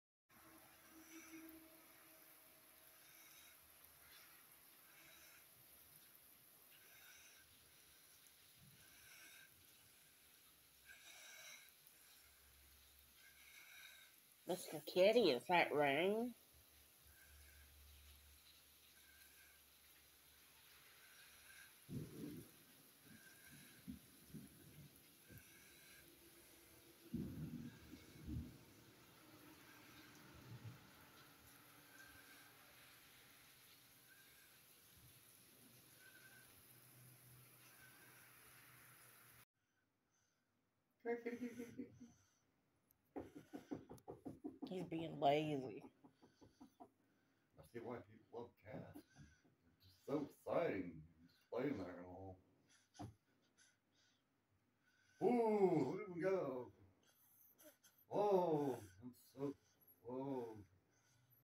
Mr. Kitty watching it rain & being lazy with Whiskers.
Mr. Kitty watches it rain.  7-24-24